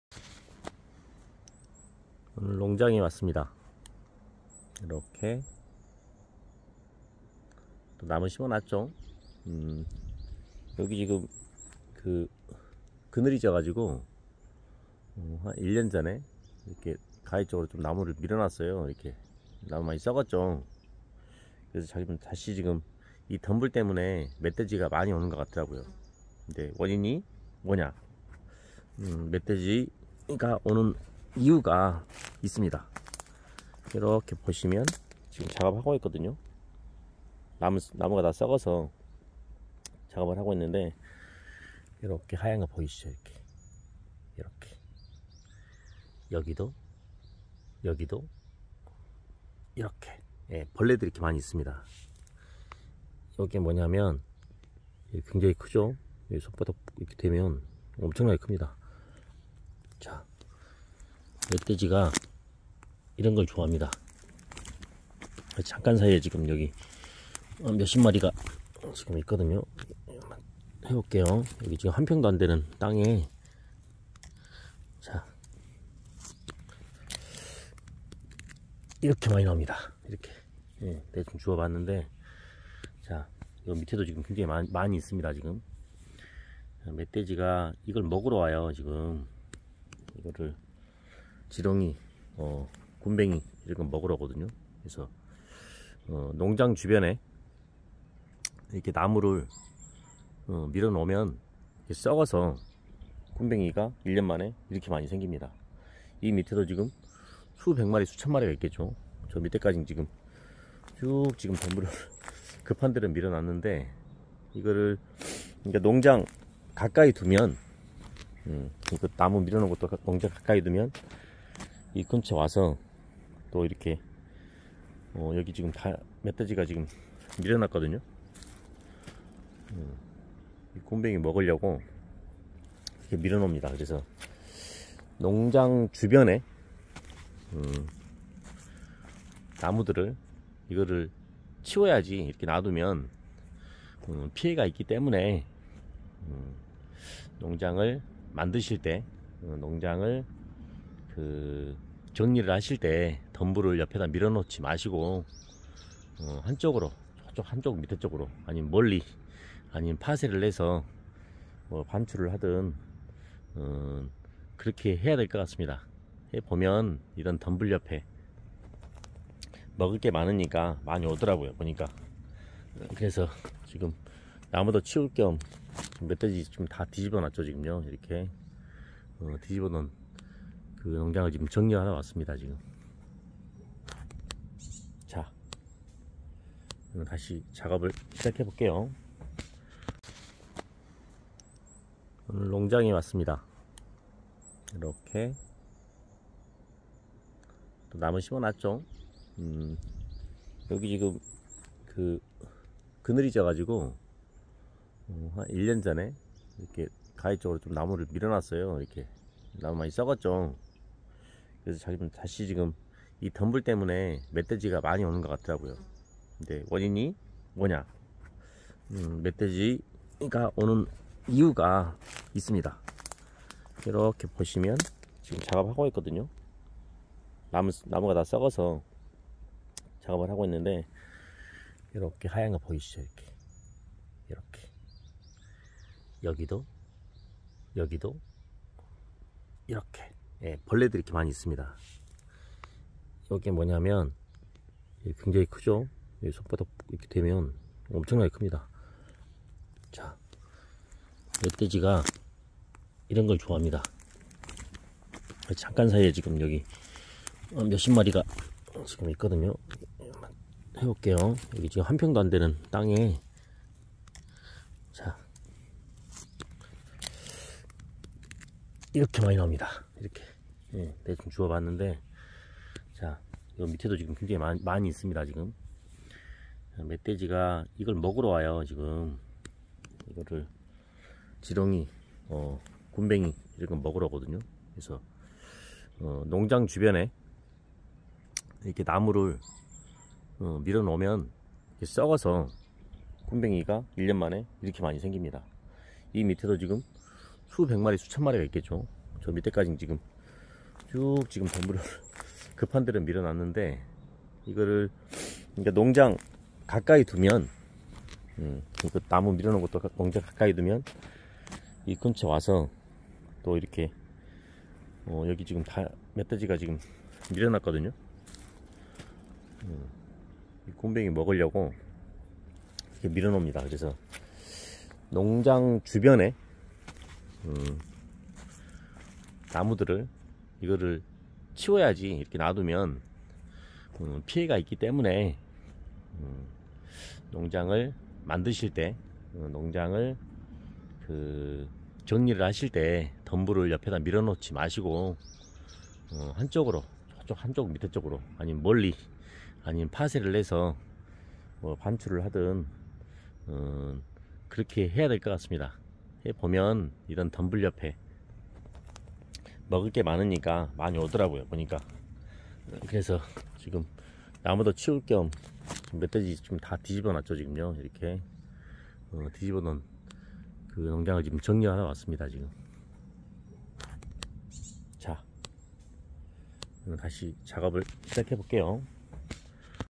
농장의 멧돼지 유인? 이놈때문
멧돼지유인하는  굼뱅이